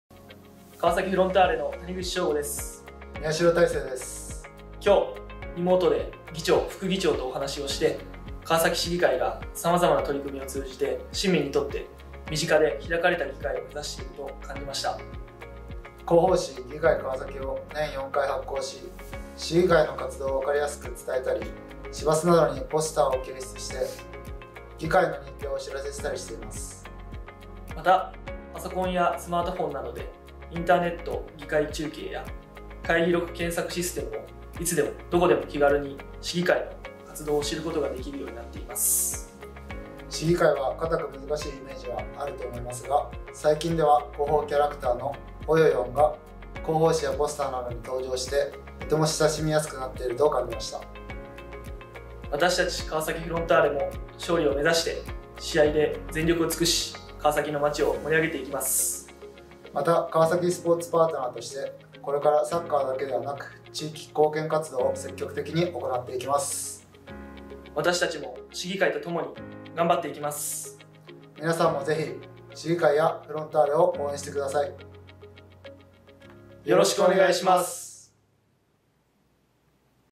「議会かわさき」川崎フロンターレ選手からのメッセージ
川崎フロンターレの谷口彰悟選手と宮代大聖選手（2021シーズンは「徳島ヴォルティス」に期限付き移籍）が、川崎市議会の山崎直史議長と花輪孝一副議長との対談を通じて感じたことなどを市民の皆さんに向けて発信します。

関連するホームページ
川崎市議会ホームページ
URL：

テキスト情報のページ
URL：

川崎市　議会局総務部広報・報道担当